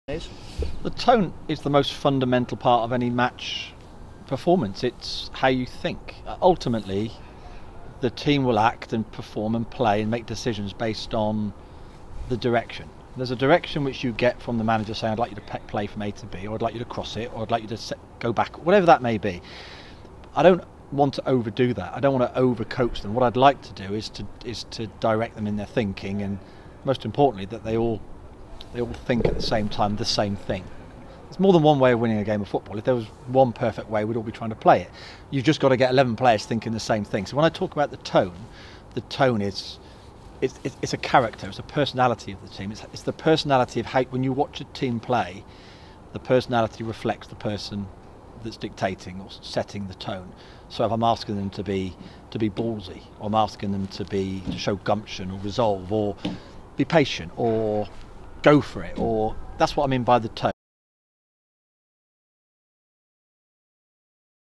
Paul Tisdale of Tis Talks TEAM on setting the tone
This video is about Paul Tisdale of Tis Talks TEAM on setting the tone for how he wants his team to play